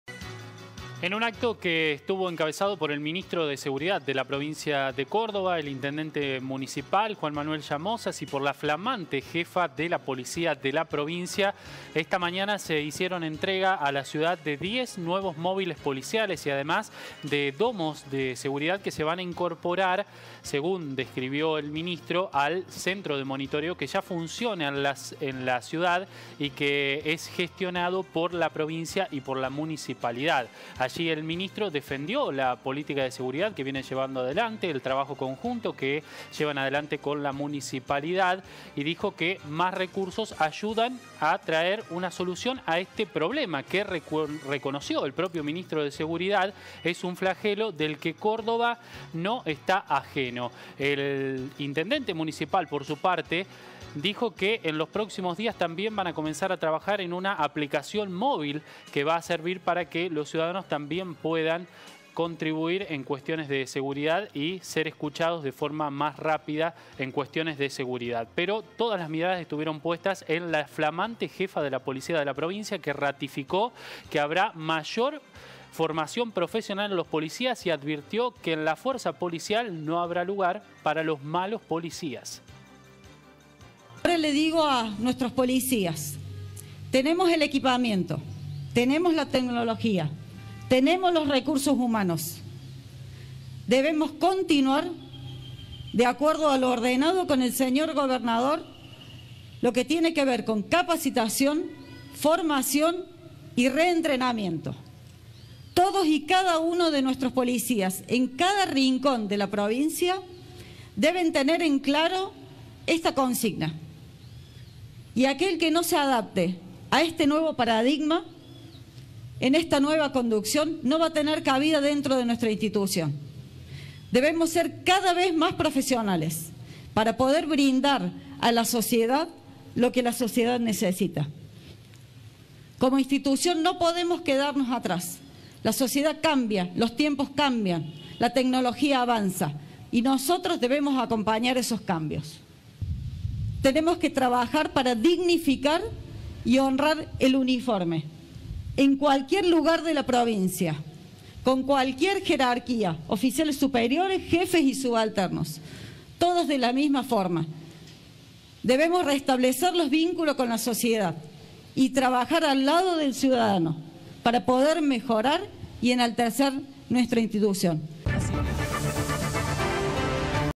ACTO POLICIA: LILIANA ZÁRATE BELLETTI Jefe de la Policía de Córdoba
JEFA DE LA POLICIA

EN LA PRIMERA SALIDA A LA PROVINCIA
LA NUEVA JEFA DE LA POLICÍA RATIFICÓ QUE LOS EFECTIVOS RECIBIRÁN
MÁS FORMACIÓN Y ADVIRTIÓ QUE NO HAY LUGAR PARA “MALOS POLICIAS”

LILIANA ZÁRATE BELLETTI
Jefe de la Policía de Córdoba
RECONOCIERON QUE LA INSEGURIDAD “ES UN FLAGELO”
EL MINISTRO DE SEGURIDAD Y LA NUEVA JEFA DE LA POLICIA DE LA
PROVINCIA ENTREGARON MÓVILES POLICIALES Y DOMOS A LA CIUDAD